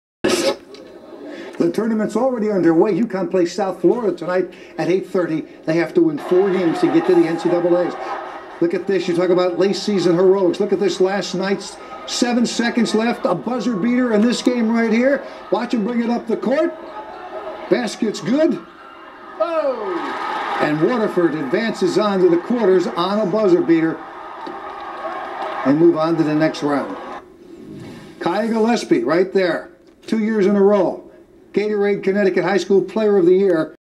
Tez TV makes local CBS Connecticut with Waterford Buzzer Beater vs Sheehan
Tez TV makes local CBS Connecticut
Waterford defeats Sheehan on buzzer beater
CIAC Class M Boys Basketball Playoffs
Waterford High School  -  Waterford, Connecticut
Game Played on Wednesday March 11, 2015
Airs on WFSB on Thursday March 12, 2015